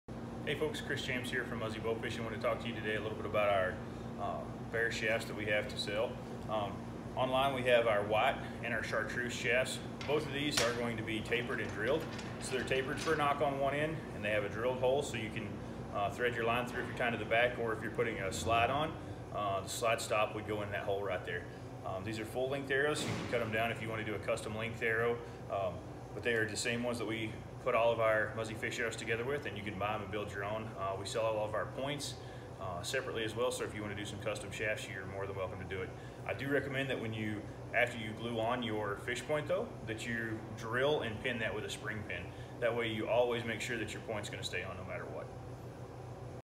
Muzzy Bowfishing Arrow Shafts
Chartreuse Fiberglass Shaft 32", bare with cross hole drilled and nock end tapered.
White Fiberglass Shaft 32” (bare w/cross hole drilled & nock end tapered)